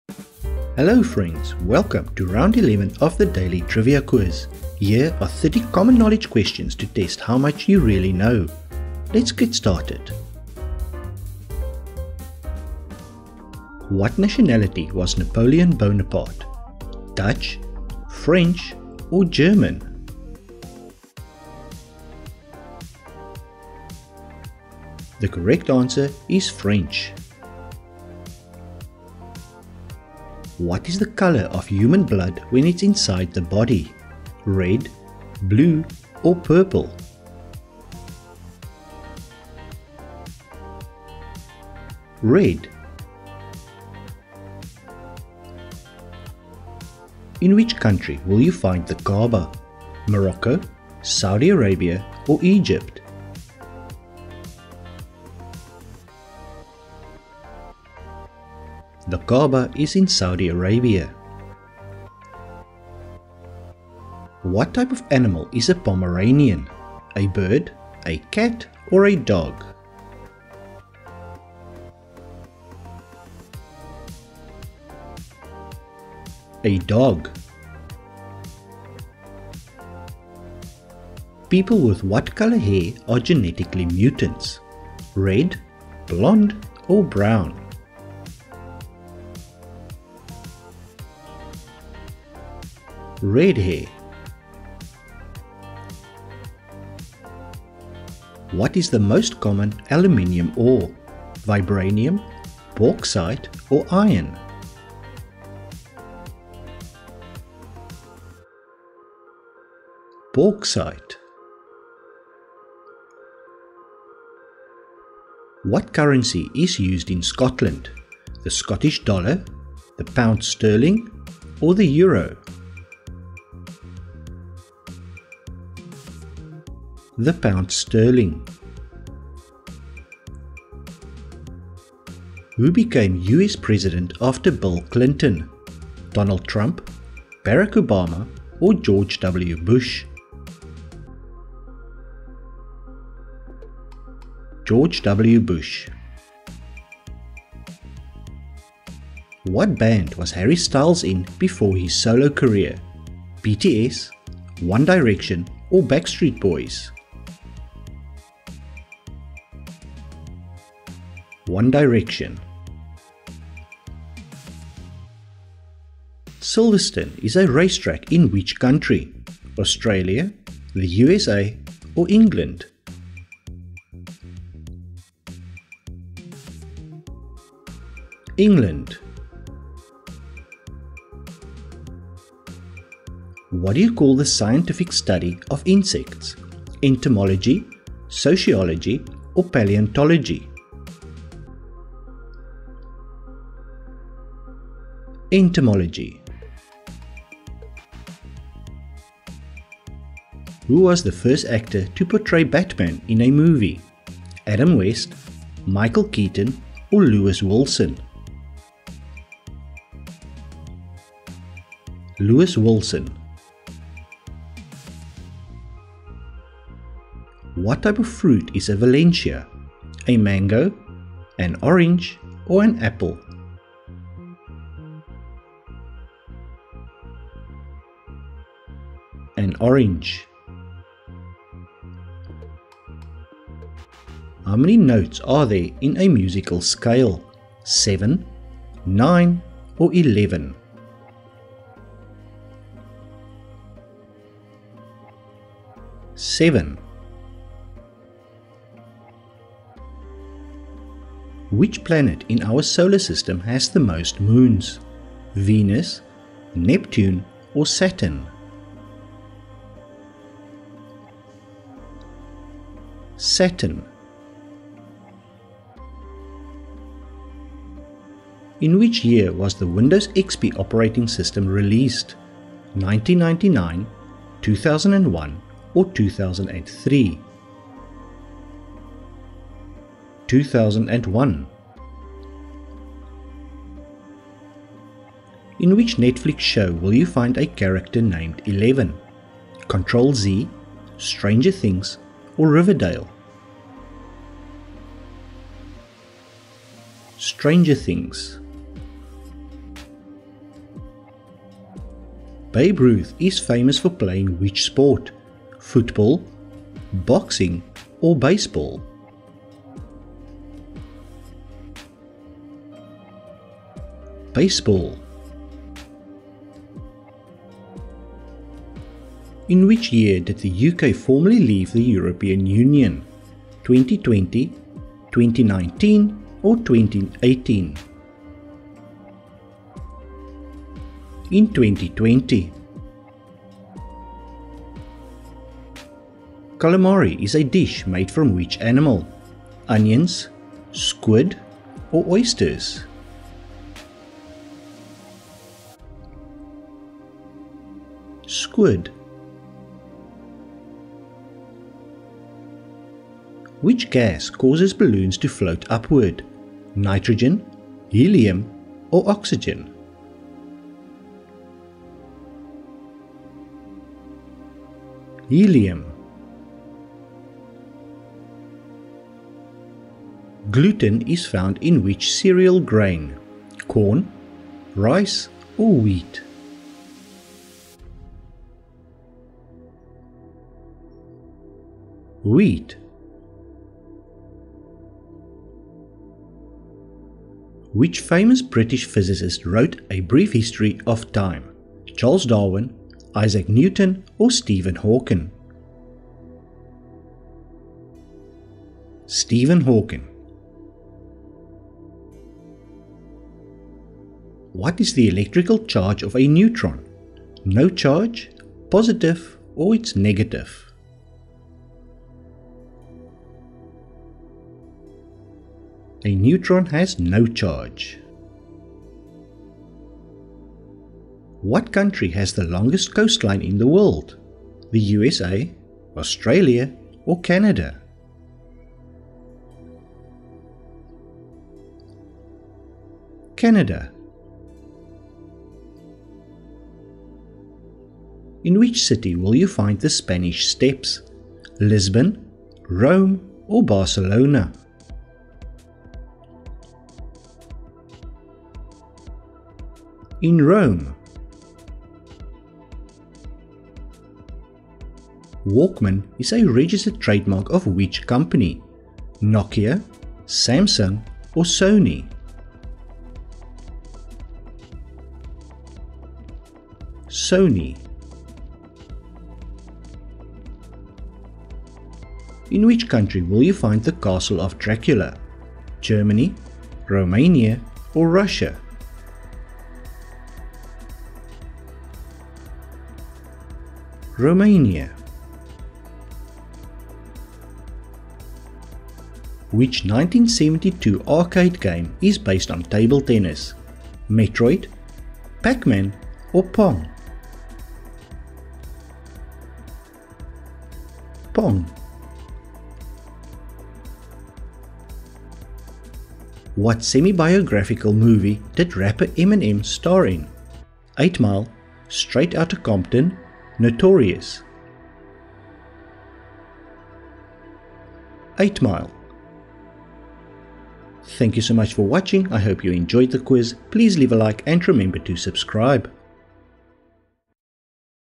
Are you Oblivious? Test your General Knowledge! | Trivia Quiz Game Round 11
Do you have good general knowledge, or are you oblivious to the world around you? See how smart you are with this trivia quiz game!

In this trivia challenge there is a random mix of general knowledge questions about stuff everyone should know. All questions are multiple choice which will make it a bit easier to answer. You should be able to answer at least 75% of the questions in this general knowledge quiz.

Enjoy Round 11 of the daily trivia quiz! Good luck!